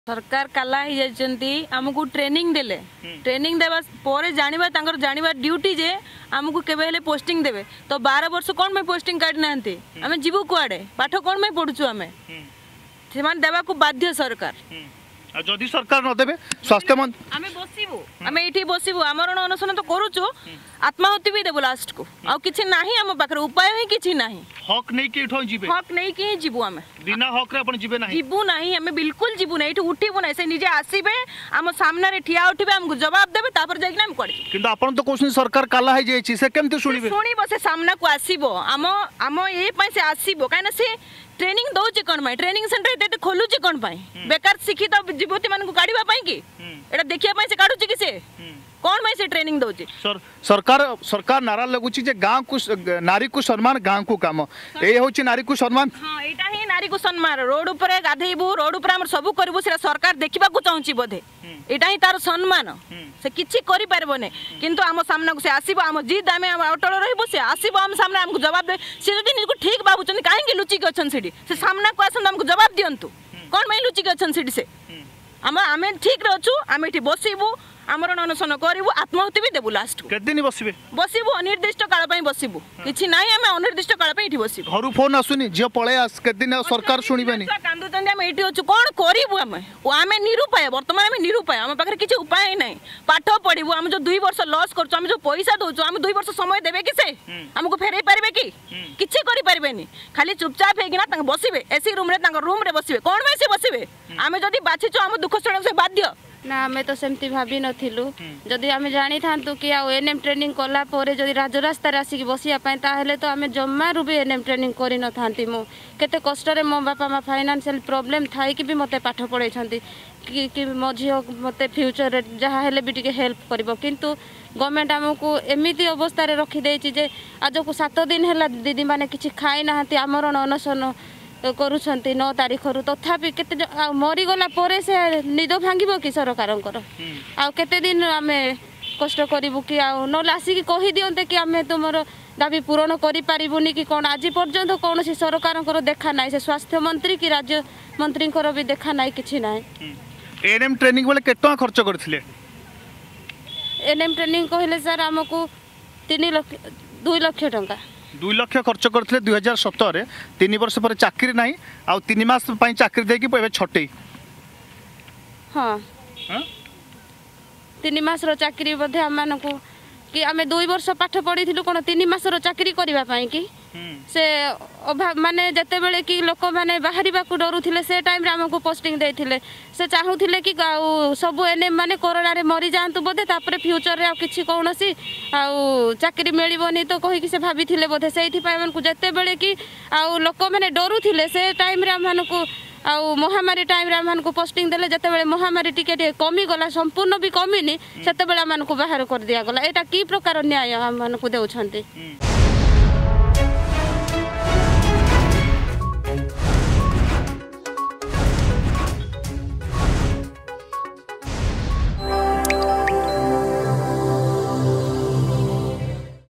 ସରକାର କାଲ ହୋଇଯାଇଛନ୍ତି
ଏଏନଏମ କର୍ମୀ ଧାରଣା : ସରକାର କାଲ ହୋଇଯାଇଛନ୍ତି...ଆମେ ହକ୍ ନେଇ ଯିବୁ ନହେଲେ ଆତ୍ମାହୁତି ଦେବୁ